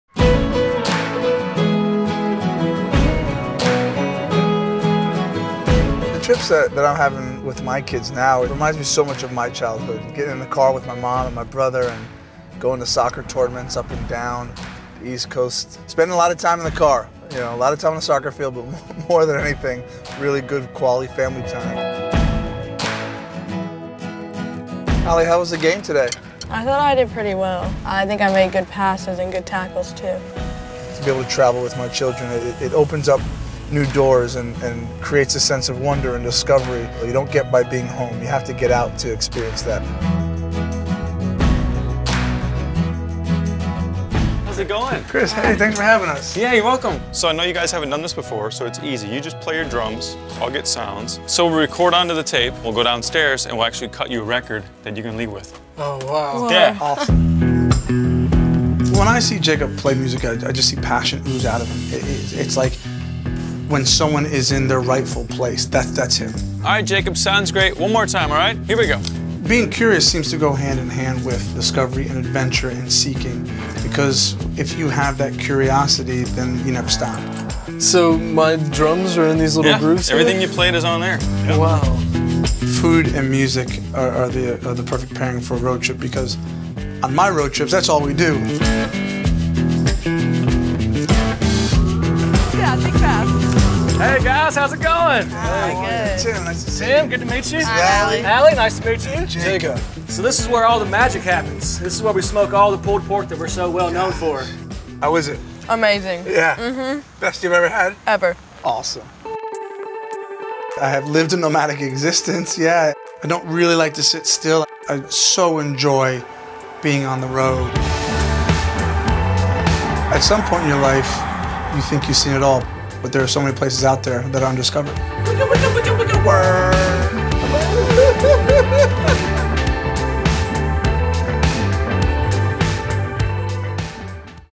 Tim Howard’s Land Rover Adventure
Rediscover music. The Land Rover Discovery takes former professional soccer player Tim Howard on a quality family road trip, exploring Nashville’s food scene and music studios with Atlas Obscura.

Learn more about how exploration brought them closer together as a family

Land Rover USA. Above & Beyond.